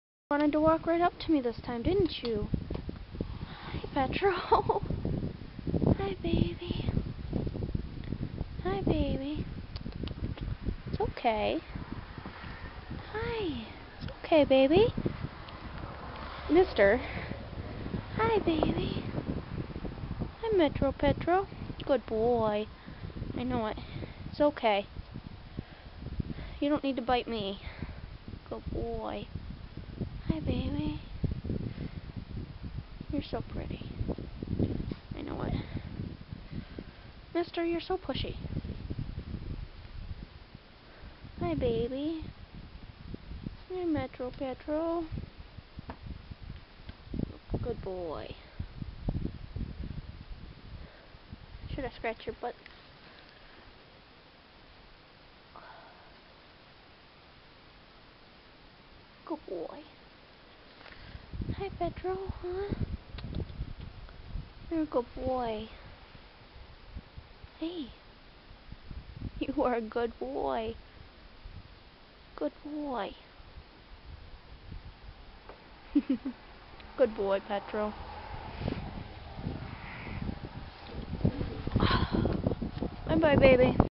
Petro, The Beaten and Abused Stallion.
Meet Petro, A year ago, this horse was given to Lisa for free...he is a $50000 classic Paso Fino stallion. He was nurotic and very dangerus, he attaked people and horses alike. All because someone decided beating him in the head with a baseball bat was a good way to train him. After he was beaten, he was scared and mean. The owners had no idea what to do, everyone at the barn was too afraid of Petro to get anywhere near him. He was stuck in a dark stall 24/7 and had his feed thrown to him as a person walked past briskley to avoid being charged. Lisa offered to take him when she saw him. The owner agreed right away, to get him on the trailer used many men and ran him onto the trailer. Then, strait form the trailer he was run into a large stall in the barn that allowed him to look outside and right where everyone brought thier horses through. If you even walked into the barn he would tuck himself into a corner and pin his ears, if you got too close he charged at you then retreated to the corner, if you brought a horse past he would scream insainley and go crazy and wasn't sure weather he wanted to kill the horse or breed it more. He couldn't tell the differnce between a gelding and a mare because he had never been socialized. After awhile of being in the stall he finally started to settle, but that is an overstatment. Lisa sowley started to be around him more and he got used to her and other people that were in the barn alot. I wont lie. I hated this horse so much, he was mean, noisey and unsettled our horses. After awhile of him getting used to lisa handleing little by little, she managed to get a lead rope on him. Once the lead rope was around him he froze and then lead, just like any horse would, oviusley snorting and blowing at certain spookey objects, but he still lead. She brought him into the round pen so he would get to see more things, people and have room to move. It wouldn't work out, he ran himself to a lather the whole time, screaming and charging at the fence. She would put him in the round pen one day and then the stall the next. Slowley little by little she was able to get a halter on him, and he got used to horses more, she even got a pull on blanket on him and never had a problem other than doing the leg starps. He still screamed and charged a bit once in awhile if you made the wrong move, and still wanted to breed the other horses, but he was being less and less agressive. Finally, many months later she was able to put him in his own pasture. He would run the fence all day, so she had to have him in the round pen, stall and pasture a few times in one week. Finally, he started to settle down out in the pasture, she noticed one of the geldings and he were playing and loveing on each other over the fence dailey. She decided to move that horse in with Petro. At first he hopped up to try to breed, but when the gelding said "NO" he never tried it again. Petro seemed even calmer, he was standing at the round bale eating instead of paceing the line. Lisa used this to her advantage, slowly she started to introduce more and more geldings to his pasture with him, at one point I even agreed to have my own gelding in with him. I was begining to like him more. Eventually, he was put out with Mr. and Rockey. Where he is now! As you can see, even the horses that seem most far gone, CAN be brought back by letting them be A HORSE! So those of you who think "Oh stallions are awfull they can't ever be handled by anyone other than a professional trainer. they can't be around any other horses they'll kill them!' NO NO NO! Let a horse be a horse! YOU are desrtoying your stallions sanity when you cut him off from everything, people, horses and the outdoors! Petro is a perfect example. I'm a teen girl and i nearley kissed this stallion on the nose,and have before. So please...see your wrong and get some geldings into the stallions pen! Allow him to walk without a stud chain and constant yanking or a switch, let them run outside! Let other people pet him. Your only makeing him dangerus by not exposeing him! There is no difference between a stallion and a mare besides the gender. they can both breed. If you treated a mare like you did a stallion a bet you'd get a crazy wild horse too.